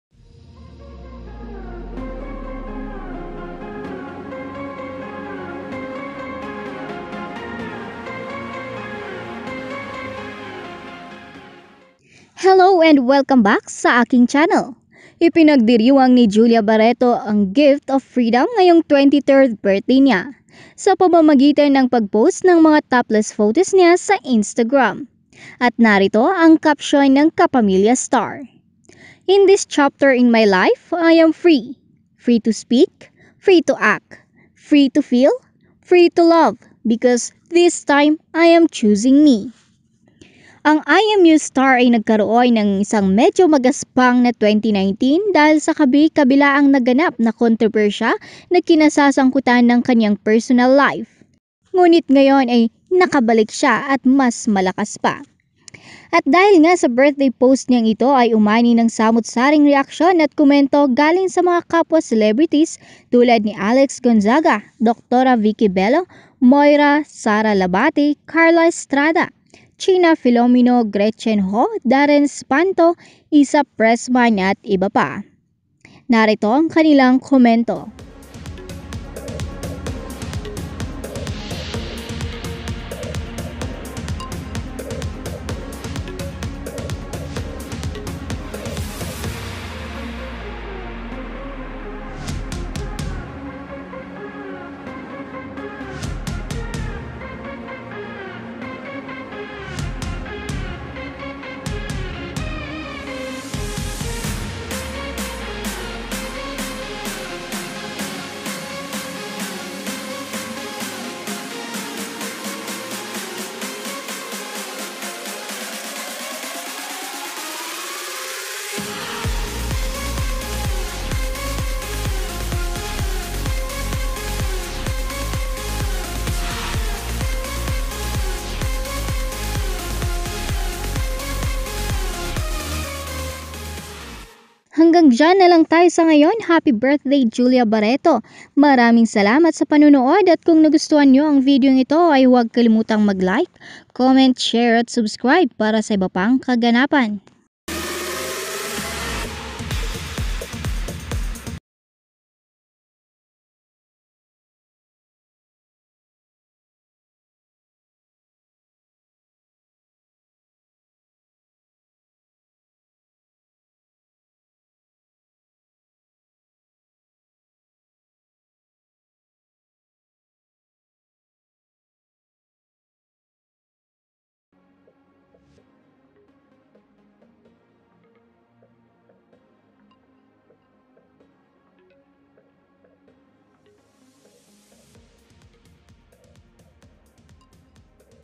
Celebrities NAGREACT sa TOPLESS birthday photo ni Julia Baretto
Ipinagdiriwang ni Julia Barretto glift of freedom ngayon 23rd birthday Niya.

At dahil nga sa birthday post Niyang Ito ay umani ng samut saring reaction at komento galing sa mga kapwa celebrities tulad Ni Alex Gonzaga,Dr Vicky Belo,Moira,Sarah Labati,Karla Estrada,chiena filomeno,Gretchen ho,Daren Espanto,Issa pressman,at Iba pa. Narito Ang kanilang komento.